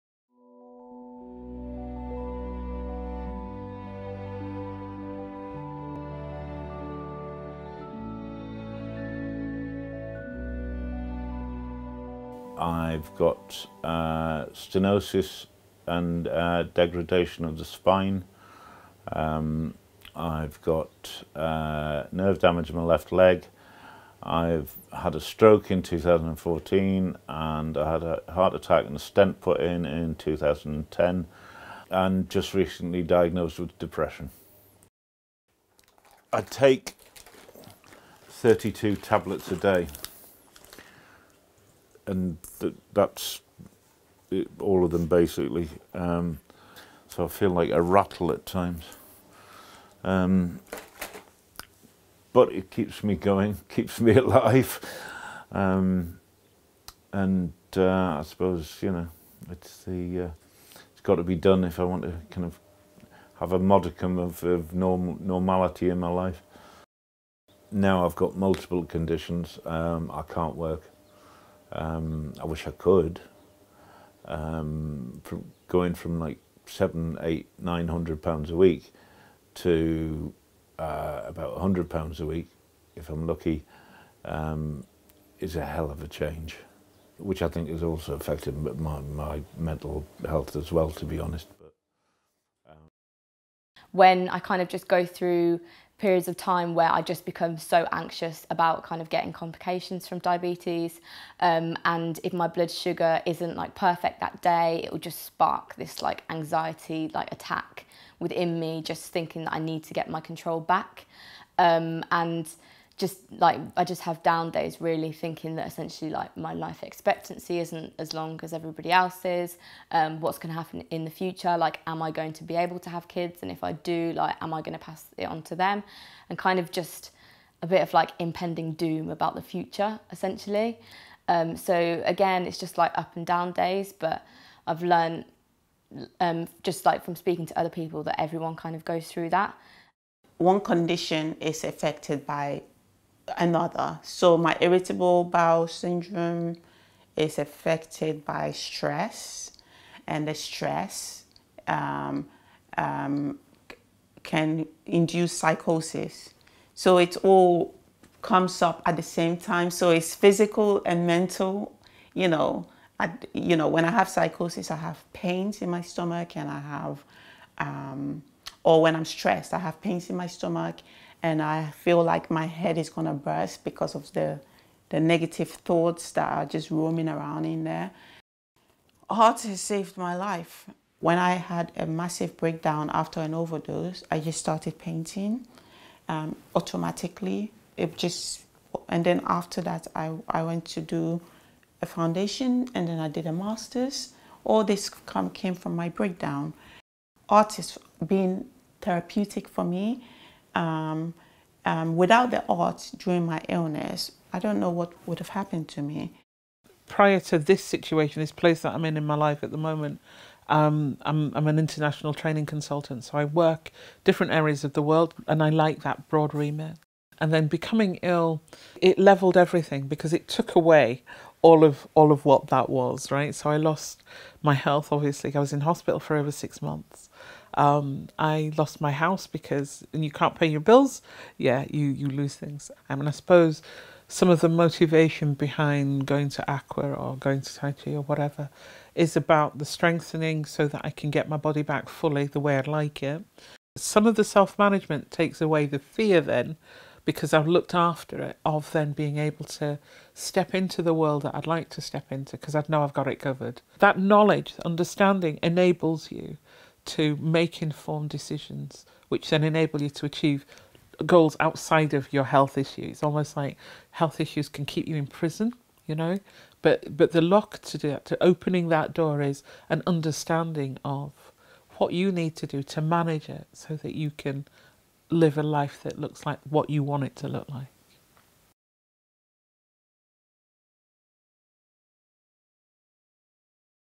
Richmond Group Multiple Conditions Mental Wellbeing Subtitled
People with multiple health conditions talk about social connections. With subtitles.